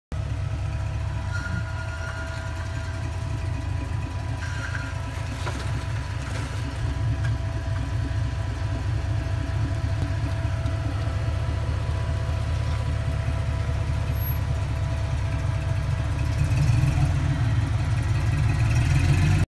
Jeep CJ5 crawling
Crawling up some rocks on long canyon trail above kyburz ca